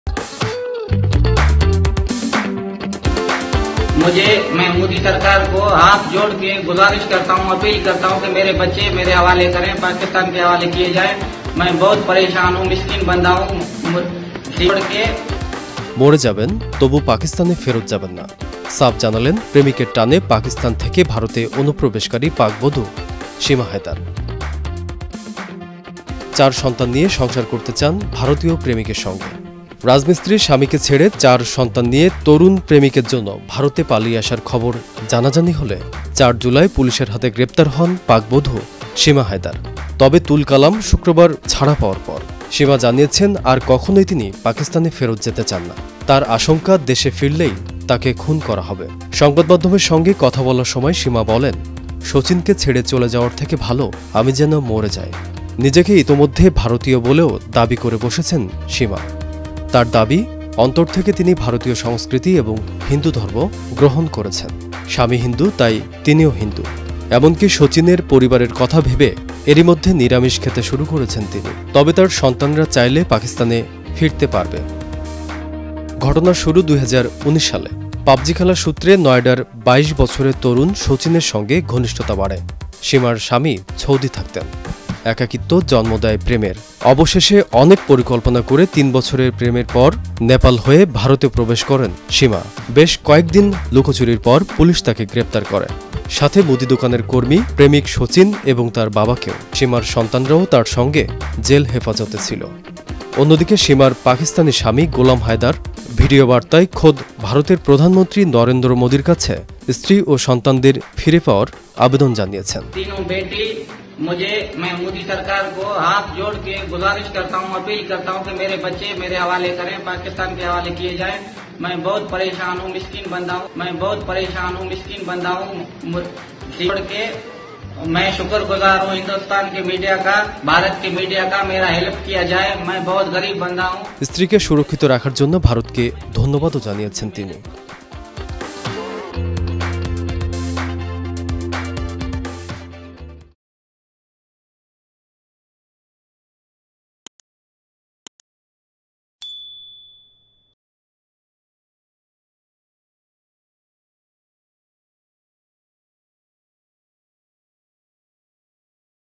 পাবজি থেকে প্রেম, ‘মরব তবুও পাকিস্তানে ফিরব না’ | Seema Haider-Sachin Meena
»» Office Address:
Bangladesh.